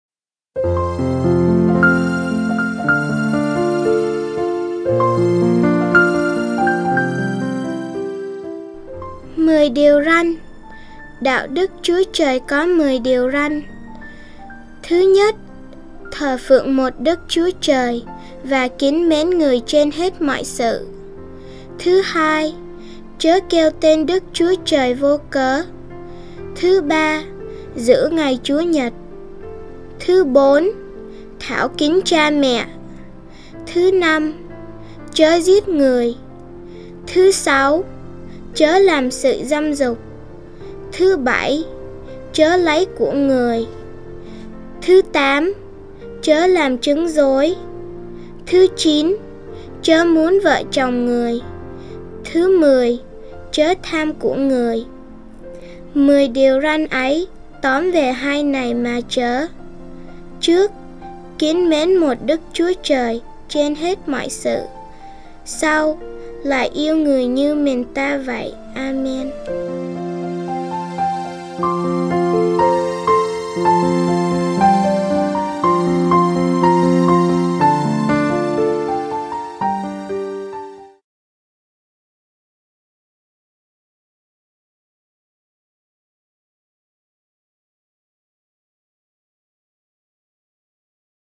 10 (Mười) Điều Răn | THIẾU NHI HỌC KINH
Đạo Đức Chúa Trời có mười điều răn:
Thứ nhất: Thờ phượng một Đức Chúa Trời và kính mến người trên hết mọi sự
Thư hai: Chớ kêu tên Đức Chúa Trời vô cớ
Thứ ba: Giữ ngày Chúa Nhật
Thứ bốn: Thảo kính cha mẹ
Thứ năm: Chớ giết người
Thứ sáu: Chớ làm sự dâm dục
Thứ bảy: Chớ lấy của người
Thứ tám: Chớ làm chứng dối
Thứ chín: Chớ muốn vợ chồng người
Thứ mười: Chớ tham của người
Mười điều răn ấy tóm về hai này mà chớ: trước kính mến một Đức Chúa Trời trên hết mọi sự sau lại yêu người như mình ta vậy. Amen.